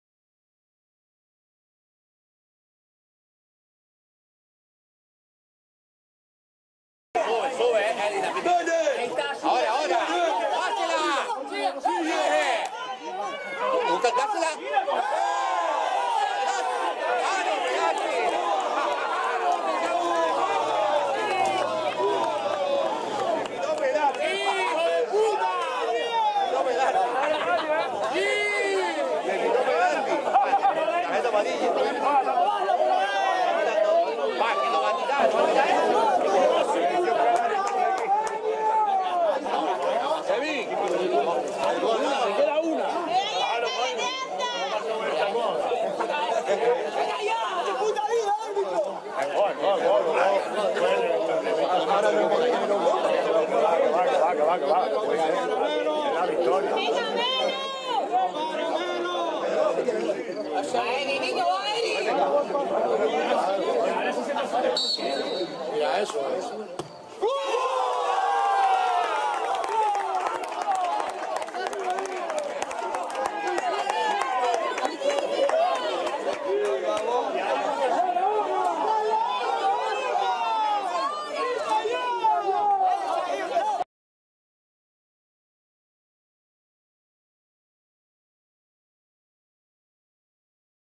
Gol de Erik, jugador del CD Vallinámar 12 2 2016
Gol de Erik, jugador del CD Vallinámar, minuto 49, de penalti, (segunda parte) 2-1. Partido de fútbol CD Vallinámar-UJ Costa Ayala. Regional Preferente Las Palmas. Temporada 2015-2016. Jornada 22. 12/02/2016. Campo de Fútbol Municipal Pedro Miranda Rosales. Valle de Jinámar. Telde. Resultado final: 2-1.